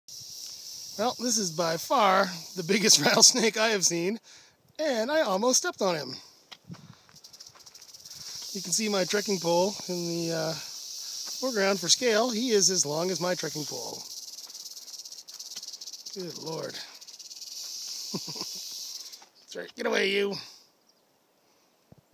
Big ol' snake
This was the biggest rattler - it was fully stretched out lying in the sun and was exactly the length of my trekking pole!